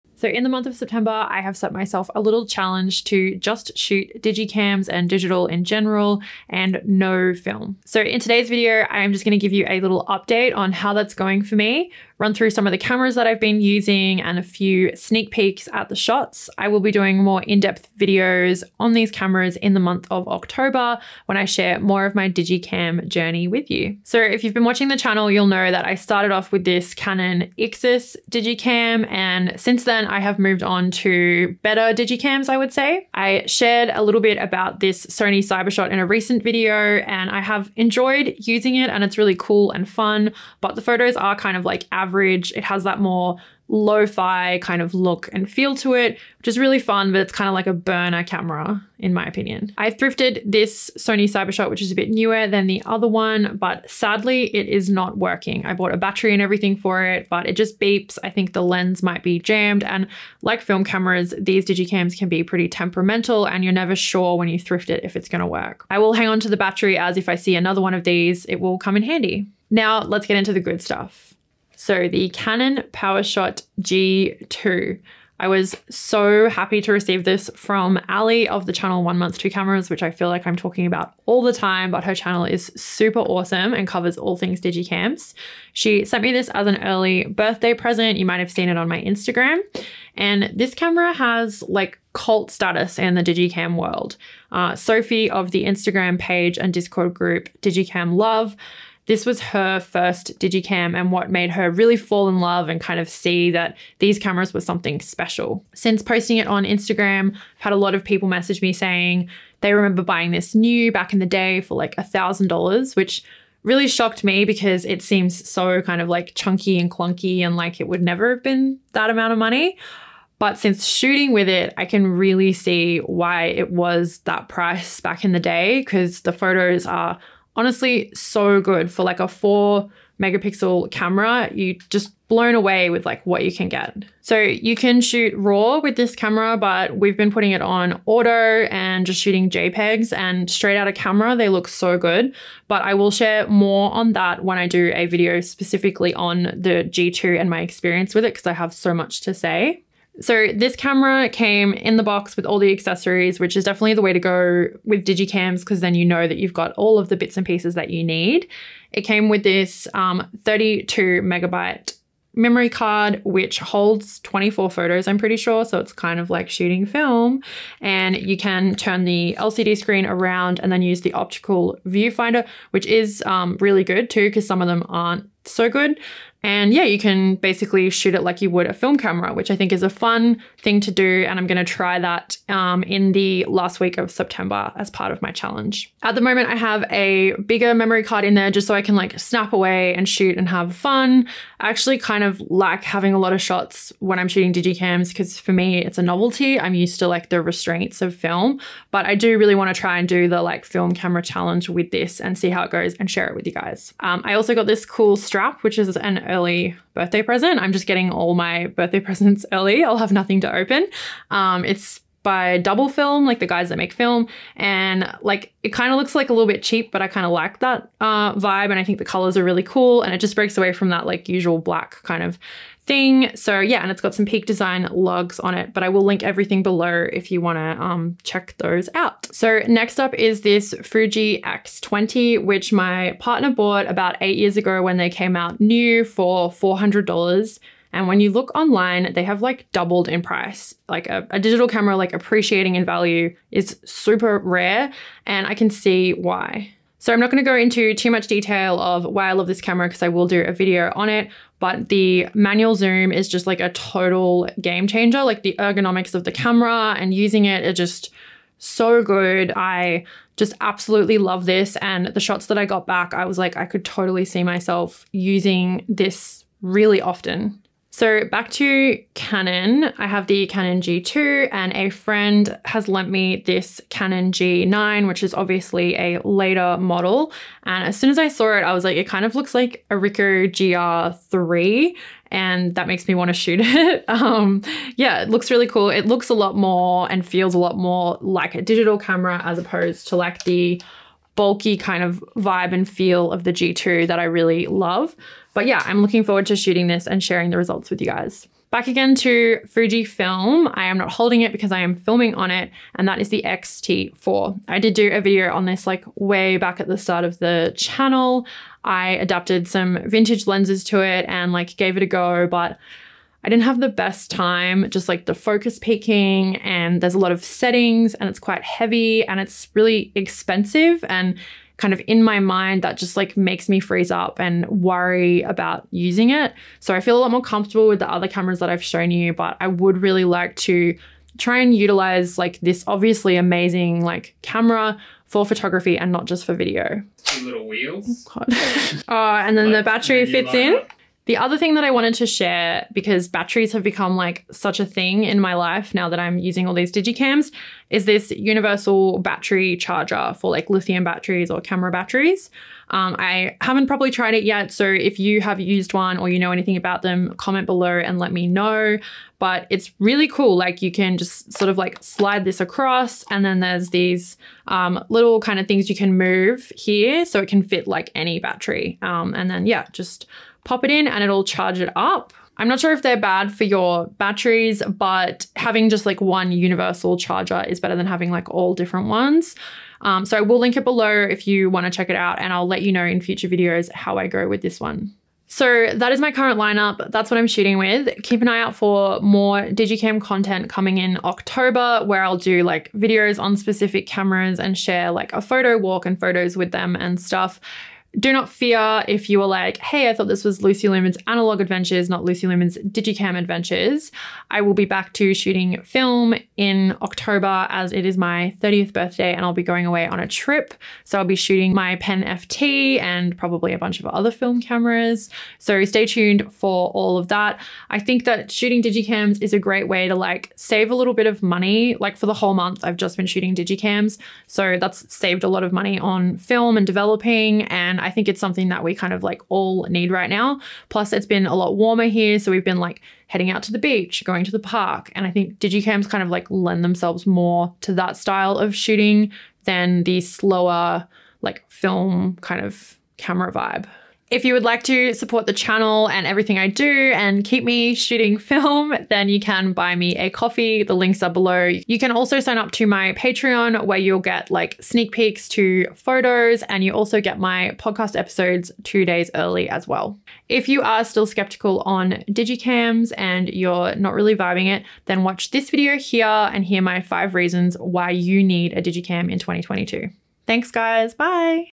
Switching From Film To Vintage CCD Digicams?
I am primarily a film photographer but have become more and more interested in digicams the last few months. I have set myself a challenge to give up film for a whole month and only shoot digicams! In this video I give a little update on the cameras I have been using and some sneak peeks of the photos. More digicam content coming in October!

Peak Design Strap Lugs
Universal Battery Charger

Browse my prints here and also start your own darkroom print shop

00:00 Intro
00:33 Sony Cybershots
01:09 Canon Powershot G2
03:48 Fujifilm X20
04:27 Canon Powershot G9
04:58 Fujifilm XT4
05:37 Universal camera battery charger
06:45 Getting back to film photography